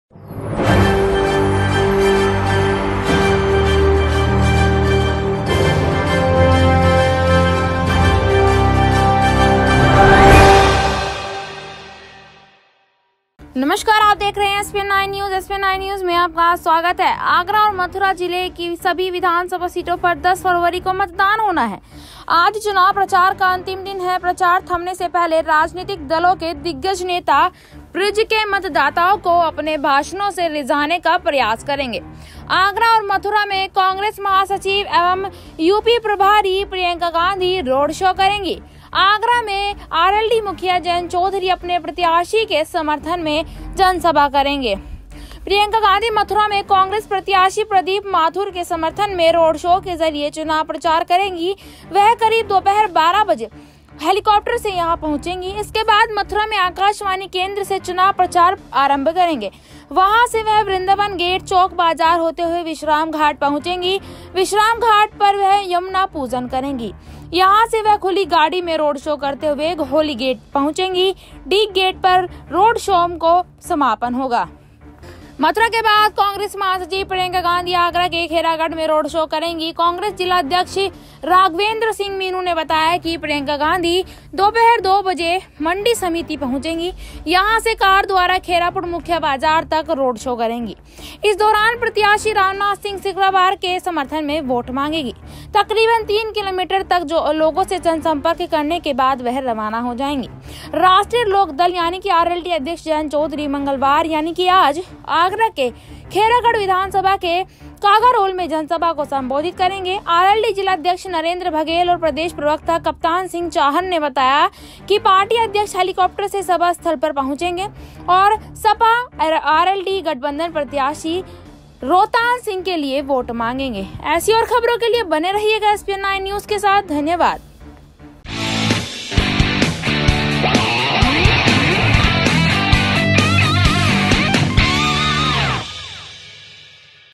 चुनाव के आखरी दिन प्रचार के लिए आगरा मथुरा पहुंचे प्रियंका गाँधी और जयंत चौधरी,रोड शो और होगी जनसभा
आगरा और मथुरा में पहले चरण का चुनाव प्रचार मंगलवार शाम छह बजे थम जाएगा। जिसके बाद जनसंपर्क और प्रचार पर पाबंदी होगी। बुधवार को पोलिंग पार्टियां रवाना होंगी। 10 फरवरी सुबह 7 बजे से शाम छह बजे तक मतदान होगा।